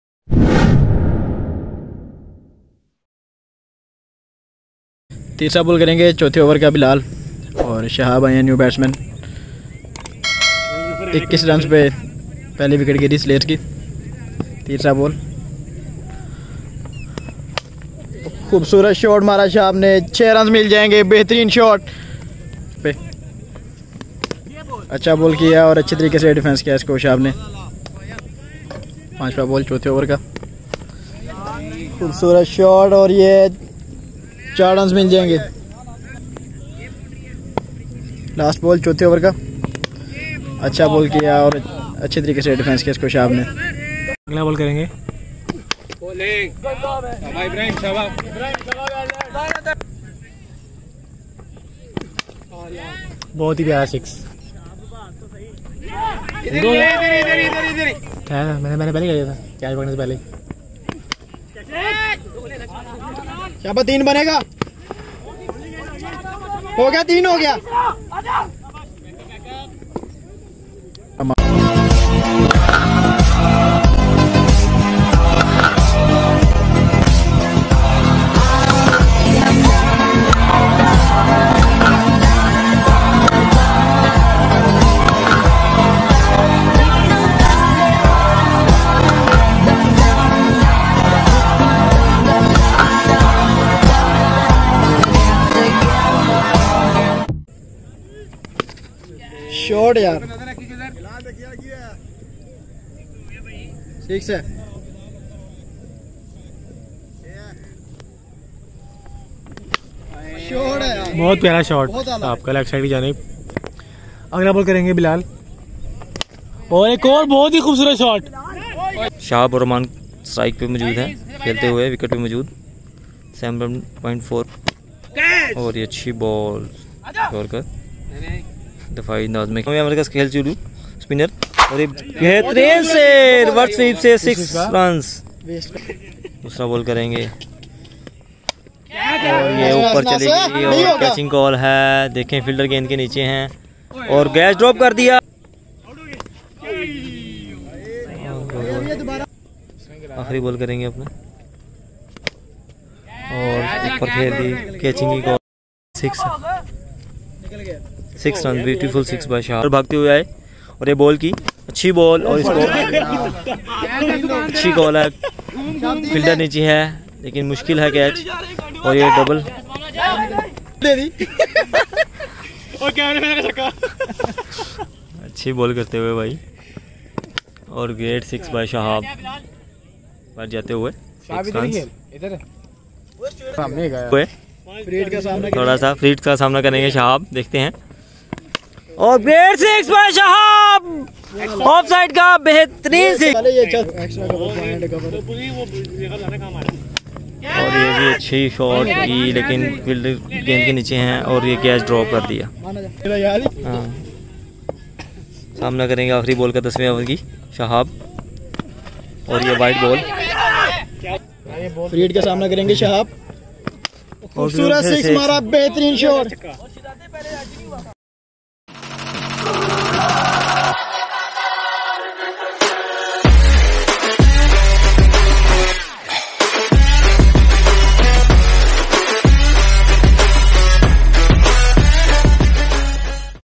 Explosive Innings by Shahab Khan against Weekend warriors
Watch the explosive Innings by Shahab Khan against Weekend warriors that helps us sets up a good target against them.